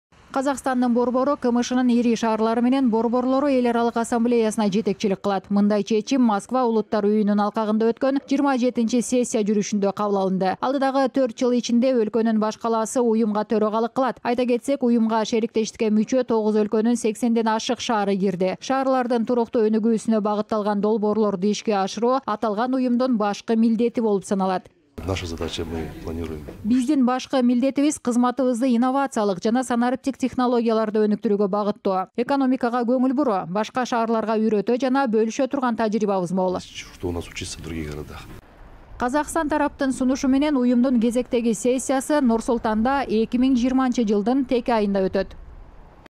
Нур-Султан борборлордун эл аралык Ассамблеясын башкарат
Казакстандын борбору КМШнын ири шаарлары менен борборлору эл аралык Ассамблеясына жетекчилик кылат. Мындай чечим Москва улуттар үйүнүн алкагында өткөн 27-сессия жүрүшүндө кабыл алынды. Алдыдагы төрт жыл ичинде өлкөнүн баш калаасы уюмга төрагалык кылат. Айта кетсек, уюмга шериктештикке мүчө тогуз өлкөнүн 80ден ашык шаары кирди. Шаарлардын туруктуу өнүгүүсүнө багытталган долбоорлорду ишке ашыруу аталган уюмдун башкы милдети болуп саналат.